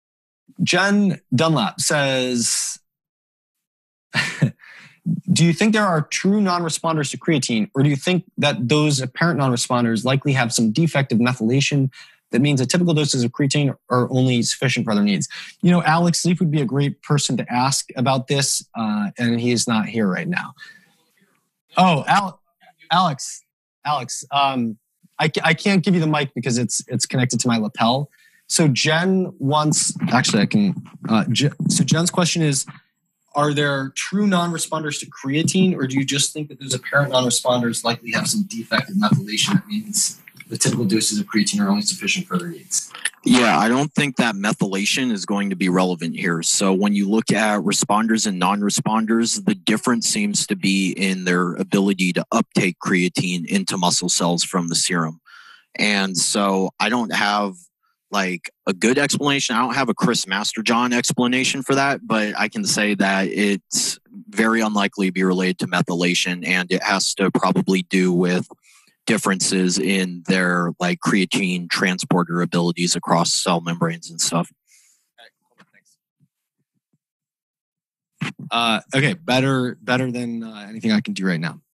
Alex Leaf answers a question about creatine non-responders and methylation.
Chris Masterjohn, PhD is the Founder and Scientific Director of the mitochondrial test mito.me. Question: Do you think there are true non-responders to creatine, or do you think that those apparent non-responders have some defects in methylation that makes typical doses of creatine sufficient only for other needs.

Alex Leaf would be a great person to ask about this and he's not here right now…

[Alex appears] Alex, so Jen's question is are there true non-responders to creatine or do you just think that non-responders likely have some defect of methylation. It means the typical doses of creatine are only sufficient for their needs.

Alex: I don't think that methylation is going to be relevant here. When you look at responders and non-responders, the difference seems to be in their ability to uptake creatine into muscle cells from the serum. So, it's very unlikely be related to methylation and it has to probably do with differences in creatine transporter abilities across cell membranes.

This Q&A can also be found as part of a much longer episode,